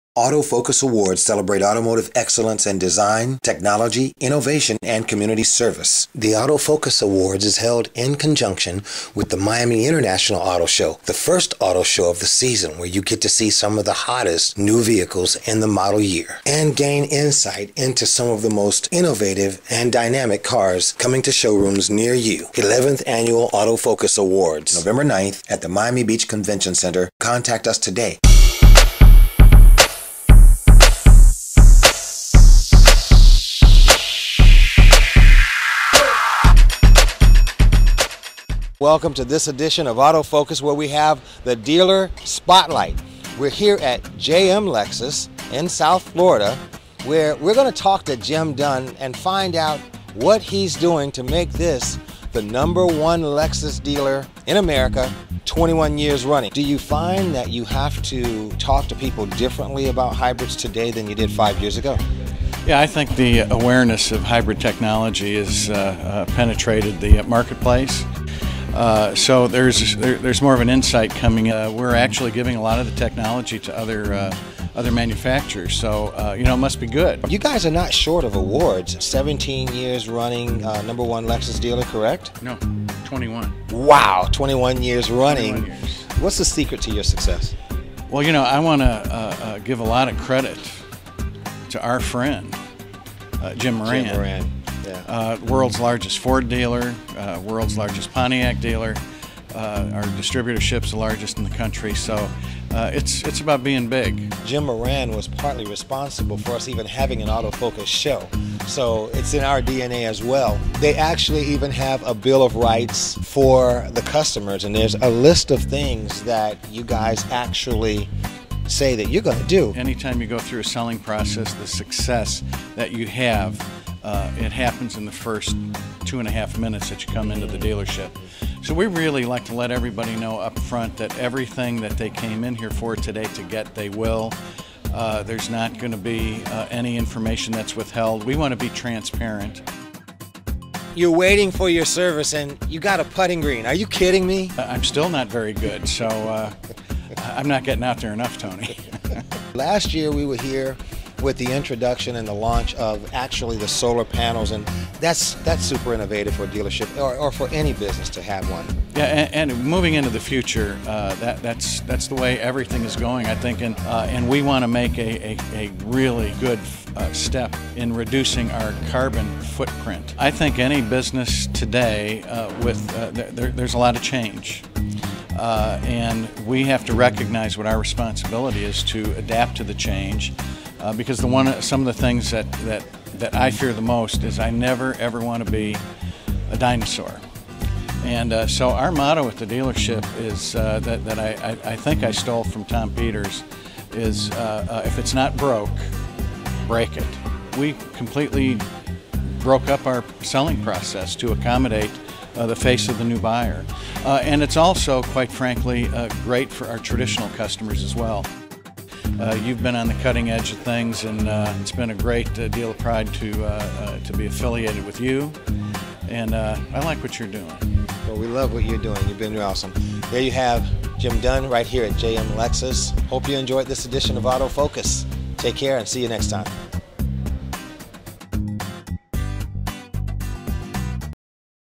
AutoFOCUS: JM Lexus show
Tony Lesesne of LMG TV visits the Award winning JM Family Lexus dealership in Margate, Florida. Jim Dunn, VP and General Manager of JM Lexus gives his inside look on why JM Family has been so succesful over the years.